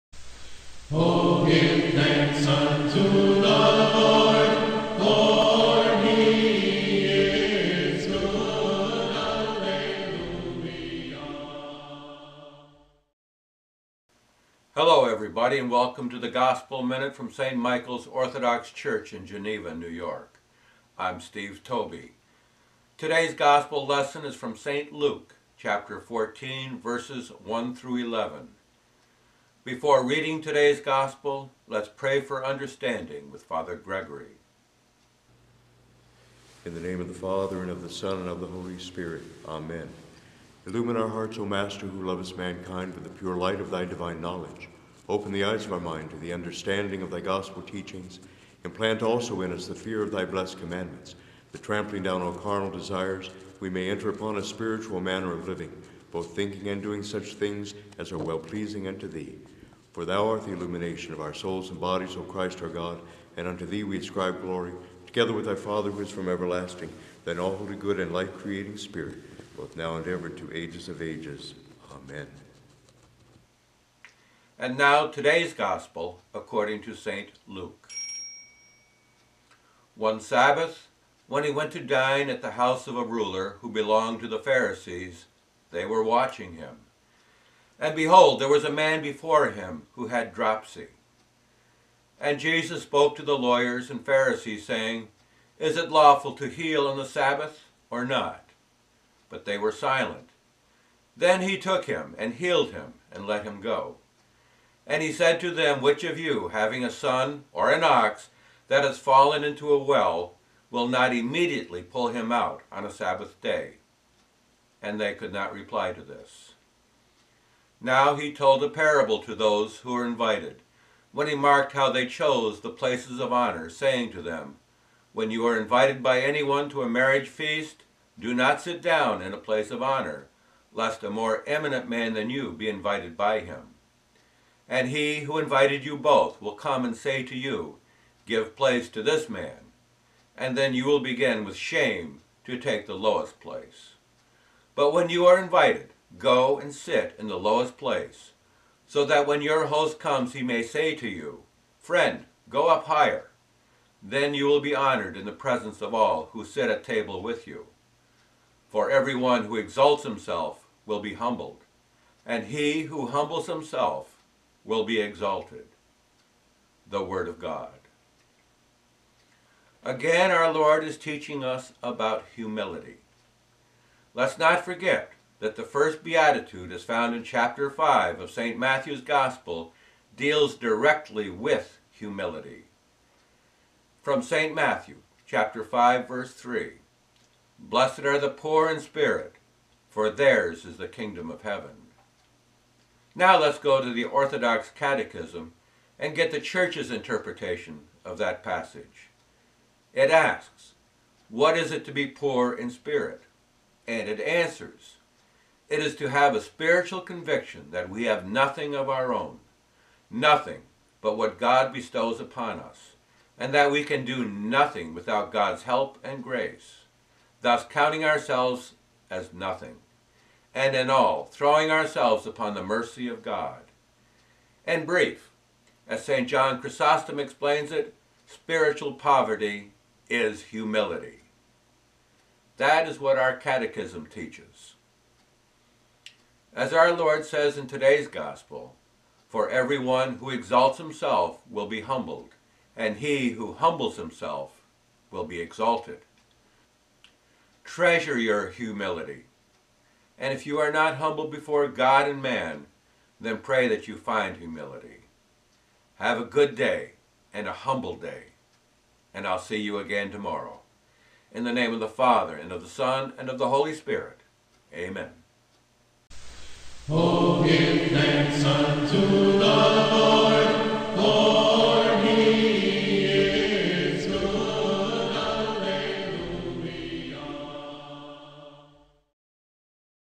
Pray We Find Humility!
The Gospel Minute for December 17th, 2016
Today's Gospel lesson is from St Luke 14: 1-11
Please share The Word Of God with your friends!